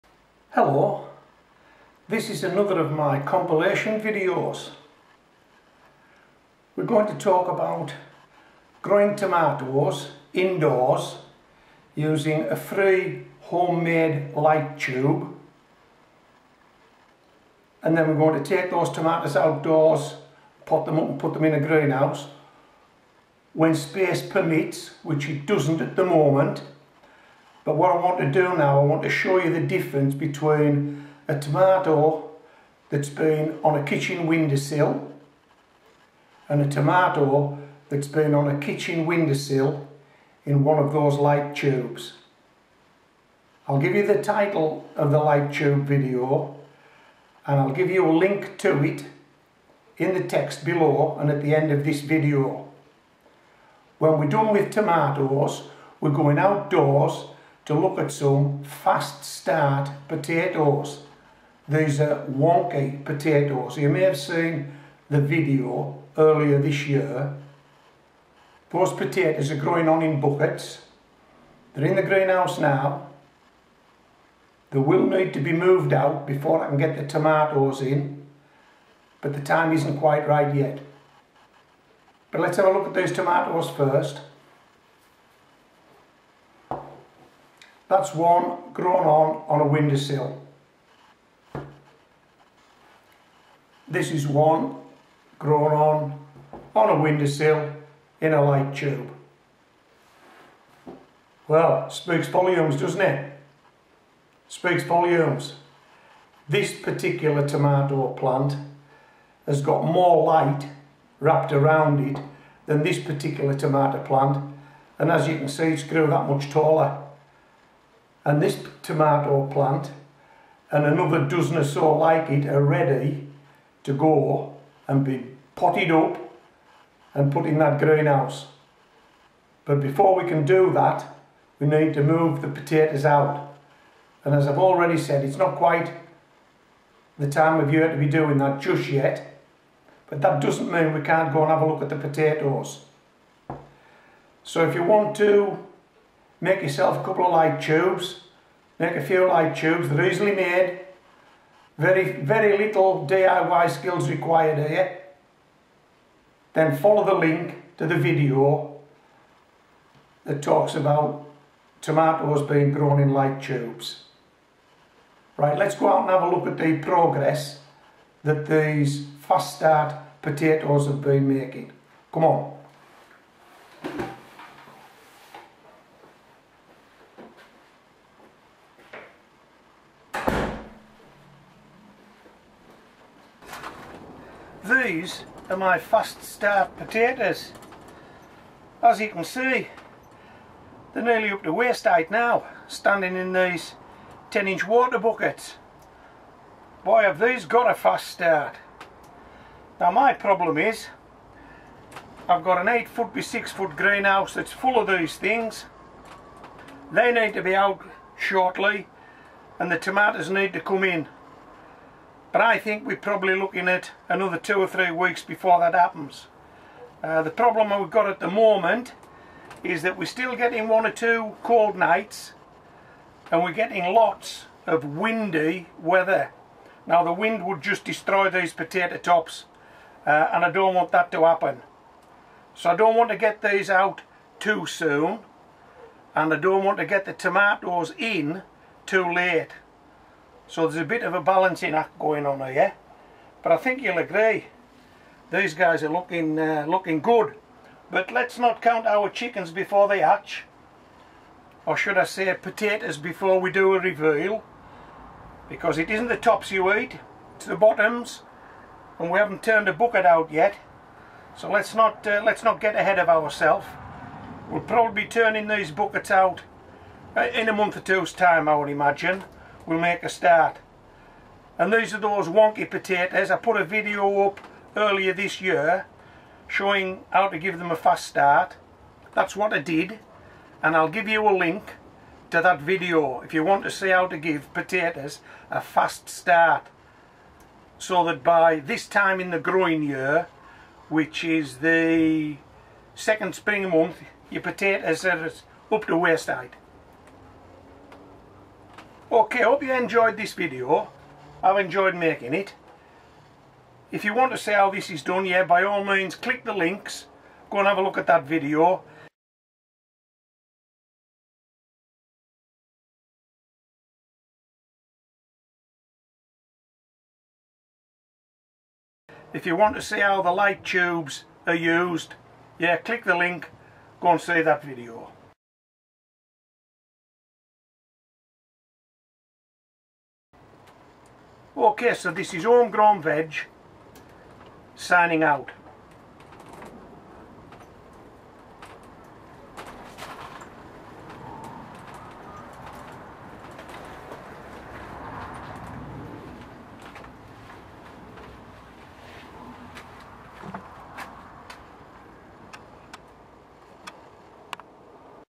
Fast start Potato update & FREE Light Tube improved Tomatoes. Ideas that work, from Home Grown Veg.
These Fast start potatoes are now nearly waist height having kicked on over the last to months in my unheated greenhouse. The tomatoes that I have been growing in home made light tubes have also made fine strong plants.
  You can find the links promised in this video in the comments below the video.